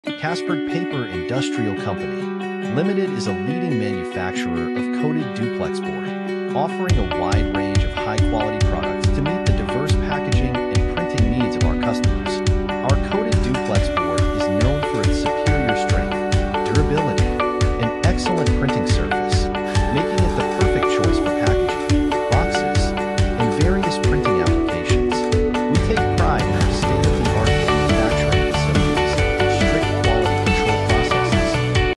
Coated Duplex Board Manufacturer
Casperg Paper Industrial Co.. Ltd. is a leading manufacturer of coated duplex board. offering a wide range of high-quality products to meet the diverse packaging and printing needs of our customers. Our coated duplex board is known for its superior strength. durability. and excellent printing surface. making it the perfect choice for packaging. boxes. and various printing applications. We take pride in our state-of-the-art manufacturing facilities and strict quality control processes.